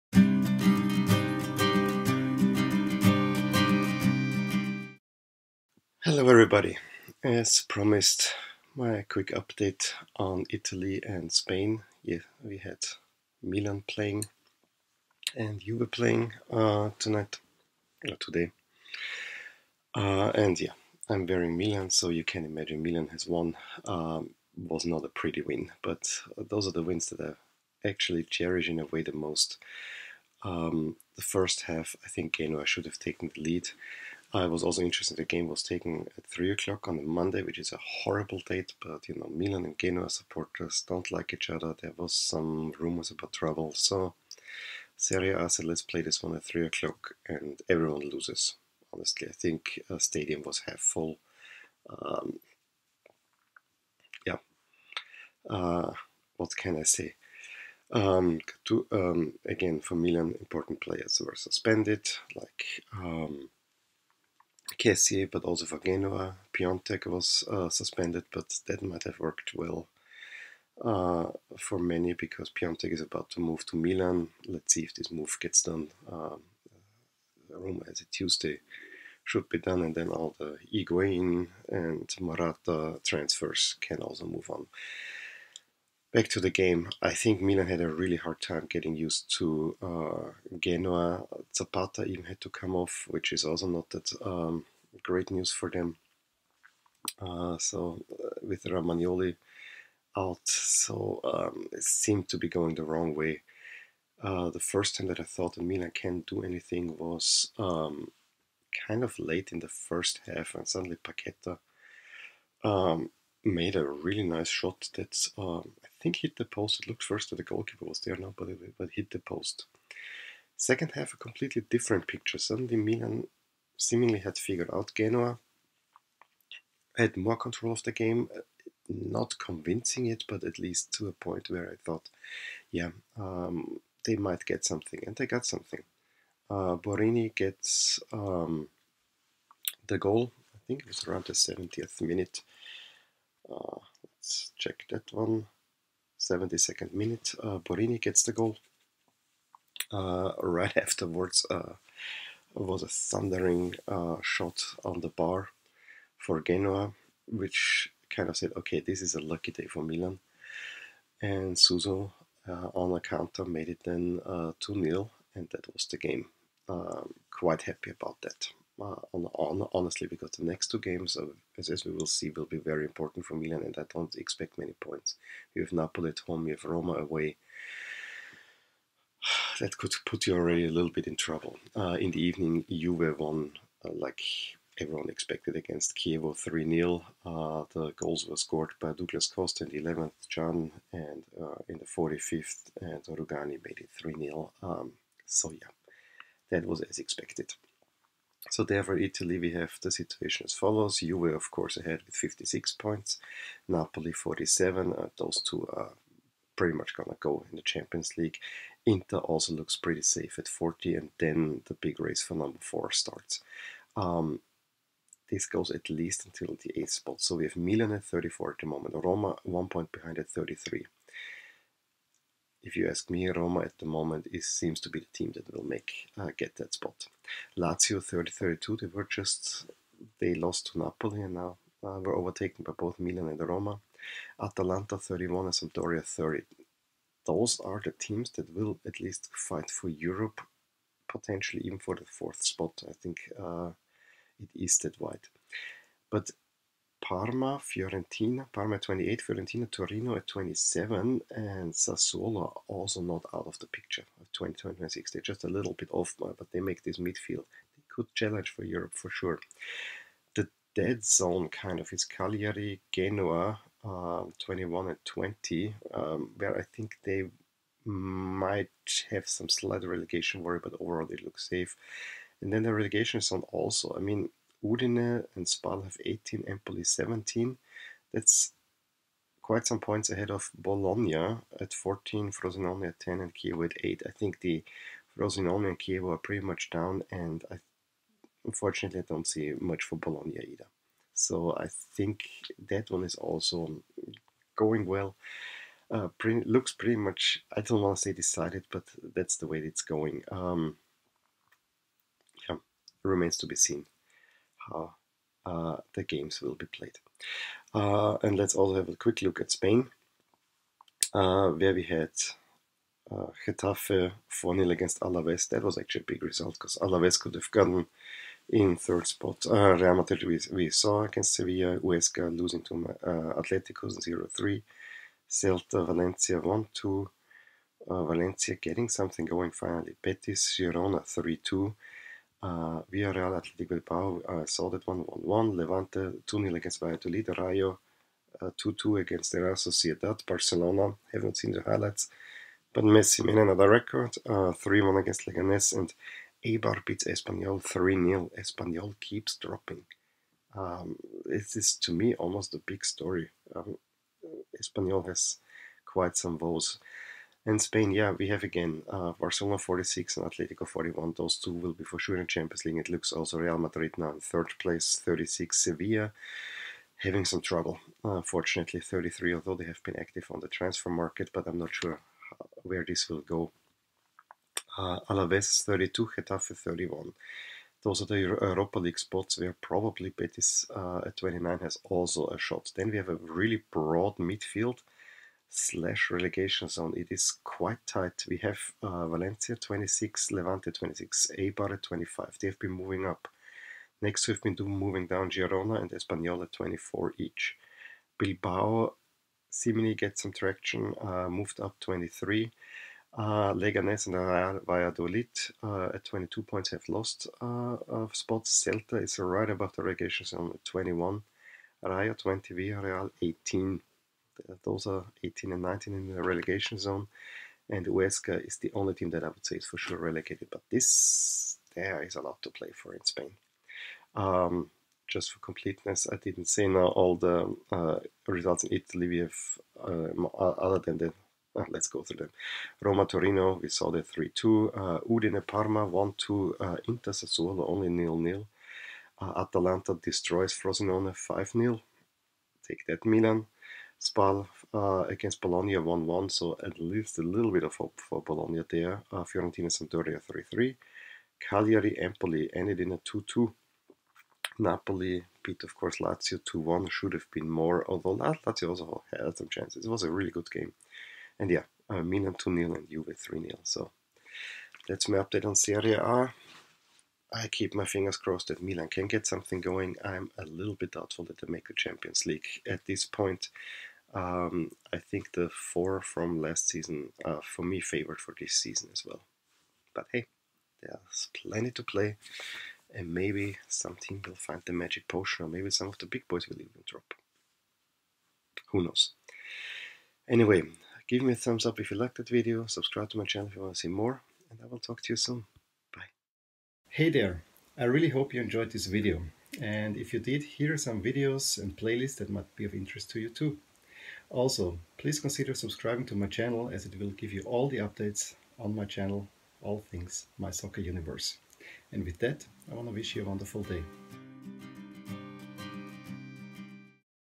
Milan With a Lucky But Important Win
It was not pretty but in the end Milan got three important points. Juve took care of business as usual. We also will have a quick look at how things stand in Italy and Spain.